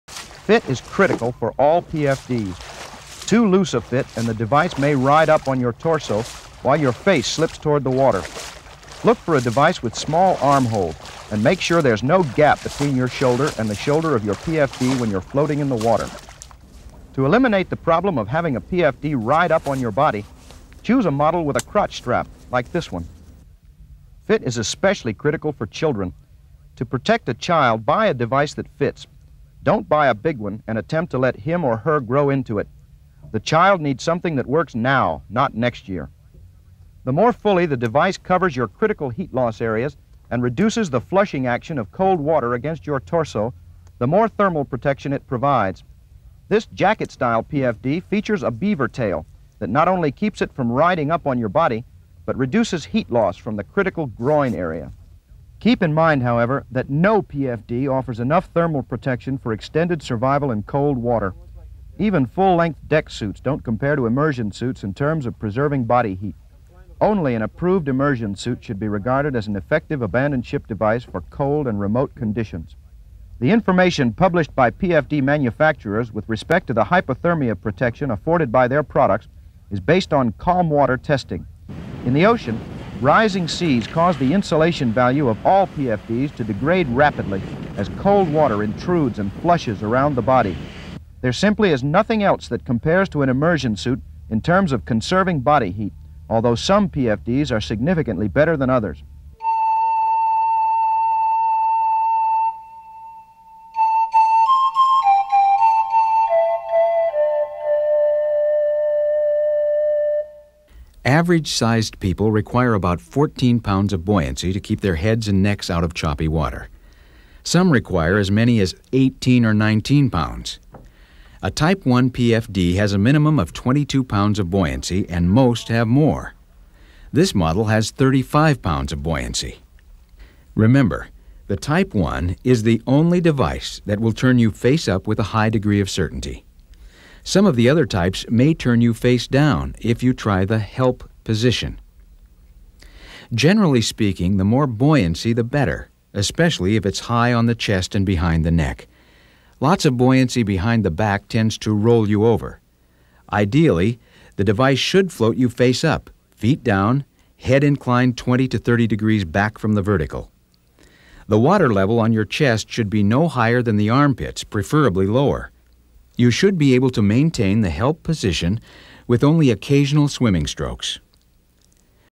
Personal Flotation Devices Trailer
No one ever plans on falling overboard, and the key to survival often amounts to whether flotation is worn all the time. With the numerous styles and types available today, there is a comfortable, wearable Personal Flotation Device that provides a significant measure of safety during virtually any marine activity if you know how to use it. This program provides a comprehensive overview of PFD types, maintenance and use. 17 minutes. DVD-Video